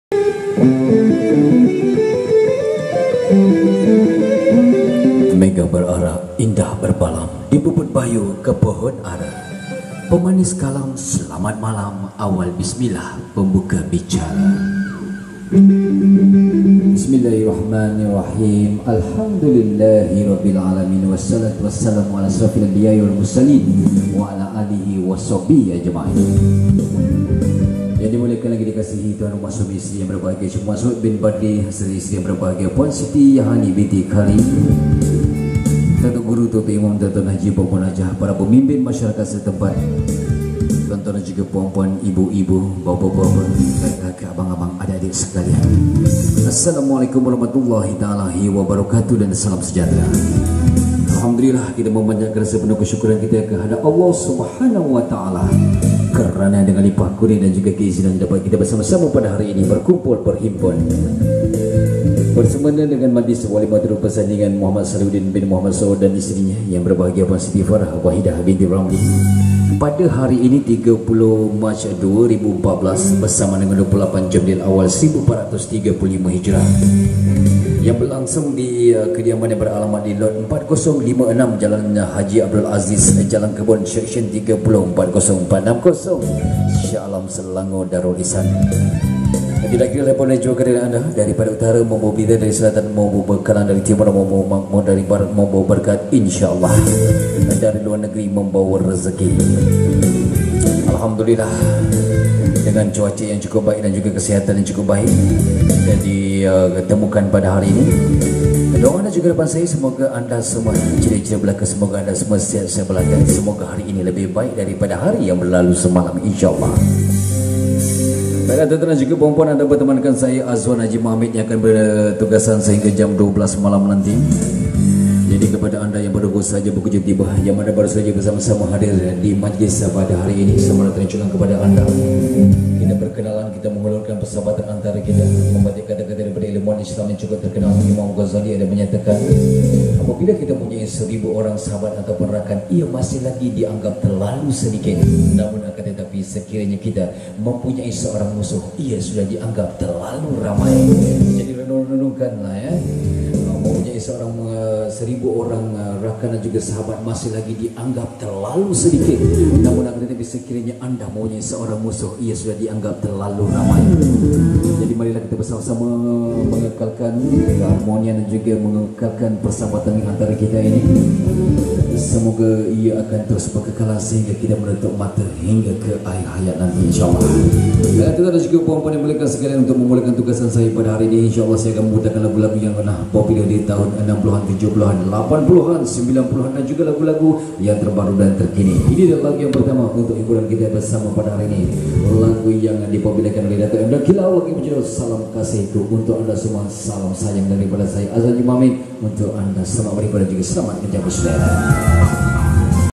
Dj Azwan Hj Mat Amid ( 0132776885 )
Sesi latihan penyampai untuk majlis perkahwinan bersama DjAzwan Hj Mat Amid ( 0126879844 ) AzwanEventSound menyediakan perkhidmatan : pa system,karaoke mobile system,Dj & emcee untuk semua majlis